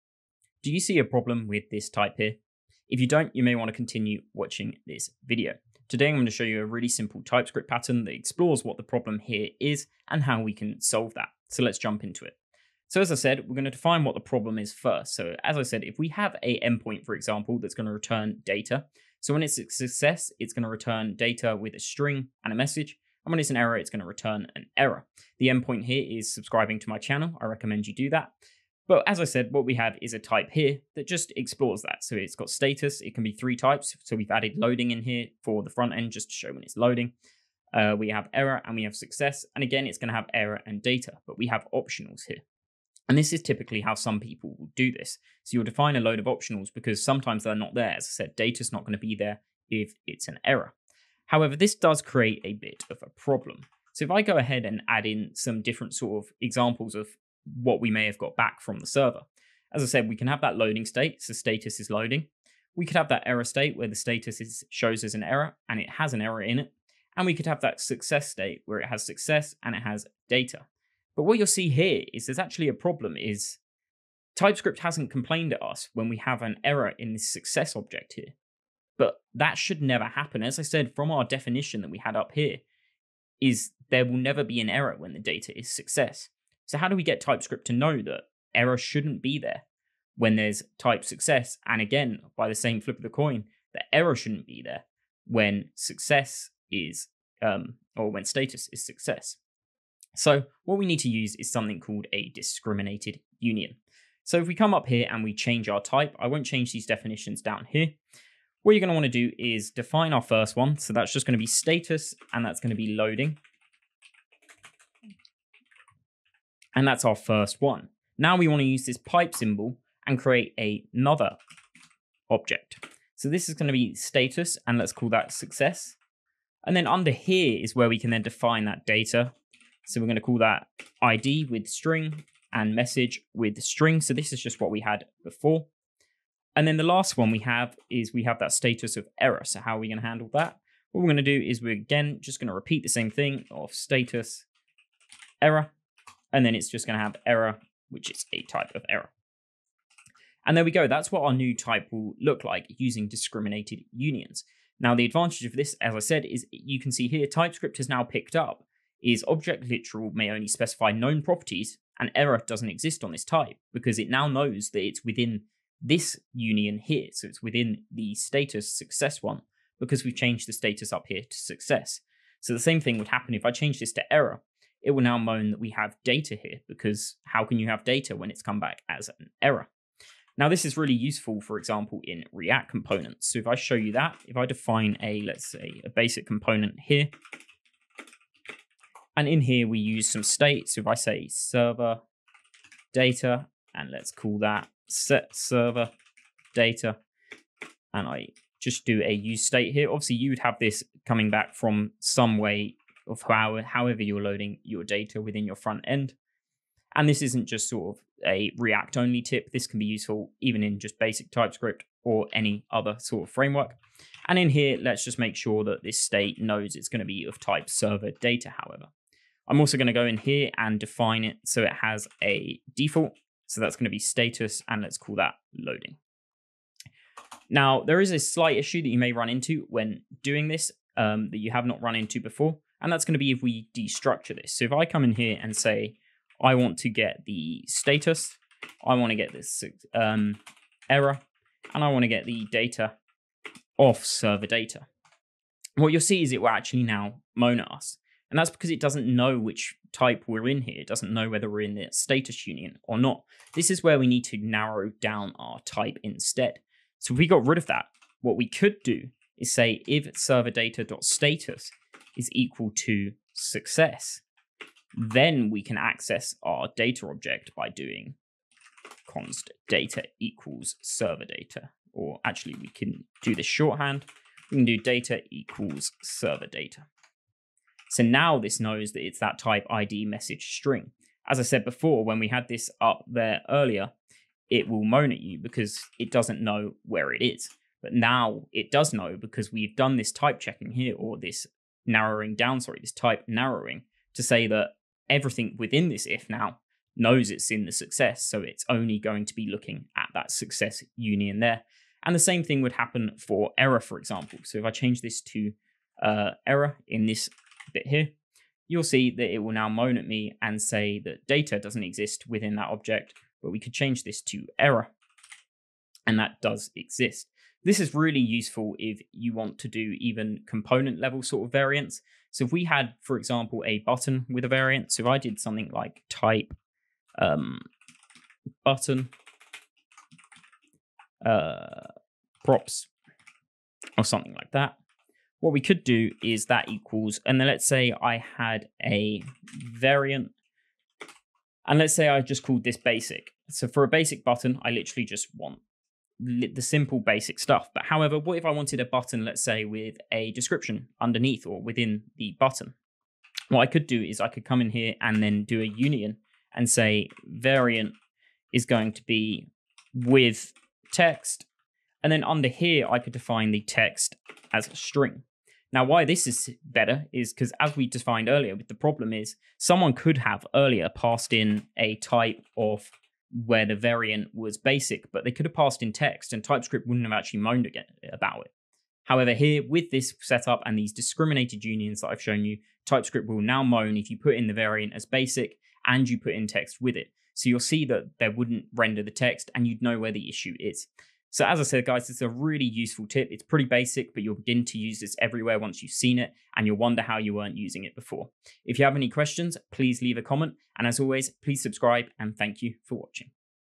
Stop using so many optionals… Simple typescript trick
A simple TypeScript pattern to conditionally include types. Useful for frontend development where you are managing state such as React props.
Discriminating unions are a simple concept that once you learn, you will use everywhere.

0:00 Introduction
0:12 The Problem
1:40 Discriminated Unions
3:00 React Component
5:35 Props Example
6:30 Outro